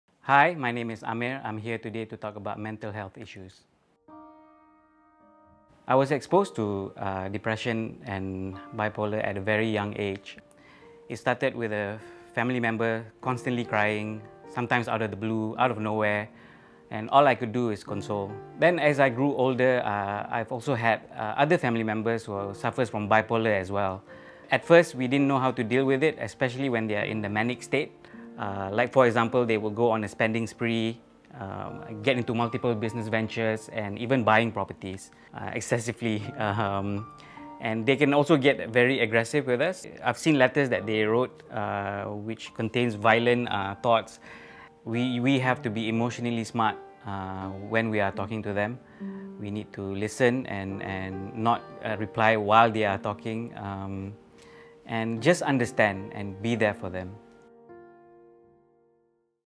My Family Suffers from Bipolar & Depression
It’s never easy watching your loved one struggle with their own mind. Find out what that’s like from Amir’s testimonial.